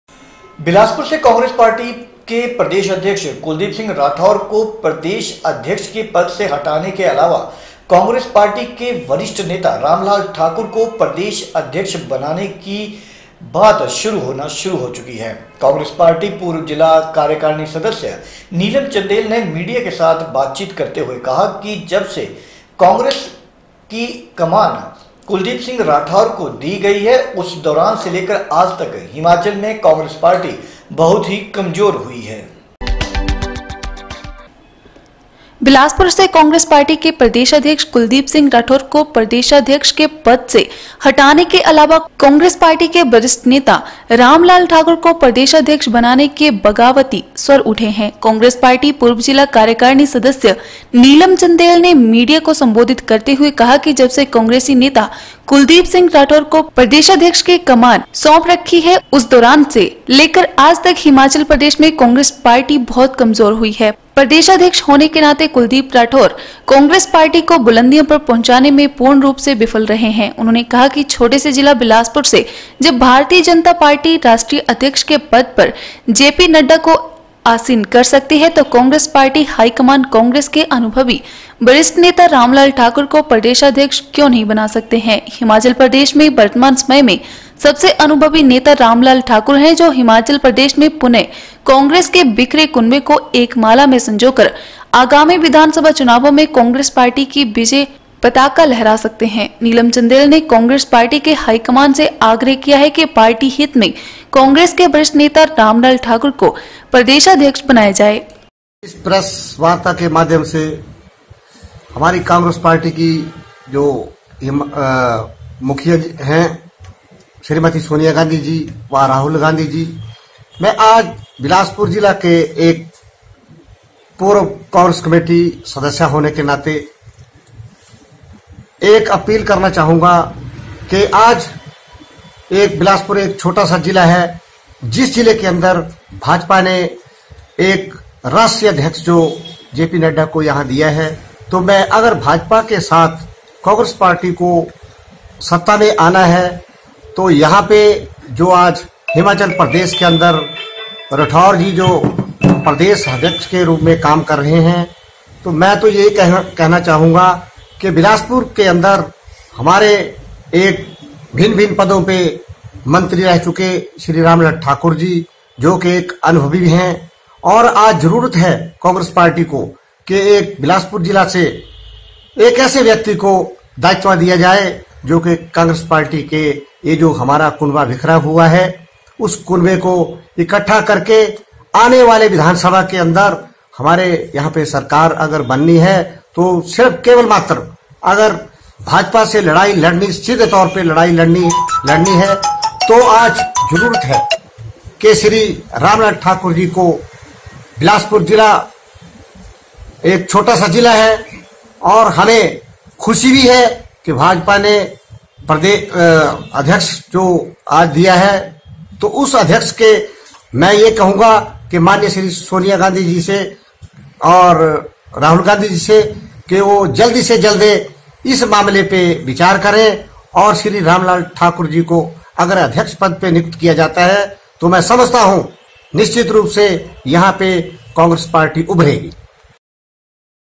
21 JAN N 5 कांग्रेस पार्टी के वरिष्ठ नेता राम लाल ठाकुर को प्रदेशाध्यक्ष बनाने के बगावती स्वर उठे
बिलासपुर से  कांग्रेस पार्टी के प्रदेशाध्यश कुलदीप सिंह राठौर को प्रदेशाध्यश के पद पर से हटाने  के अलावा कांग्रेस पार्टी के वरिष्ठ नेता राम लाल ठाकुर को प्रदेशाध्यक्ष बनाने के बगावती  स्वर उठे हैं । कांग्रेस पार्टी पूर्व जिला कार्यकारिणी सदस्य नीलम चंदेल ने मीडिया को सम्बोधित करते हुए कहा कि जब से कोंग्रेसी नेता  कुलदीप सिंह राठौर को प्रदेशाध्यक्ष की कमान पार्टी ने सौंप रखी है उस दौरान से लेकर आजतक हिमाचल प्रदेश में कांग्रेस पार्टी बहुत कमजोर हुई है।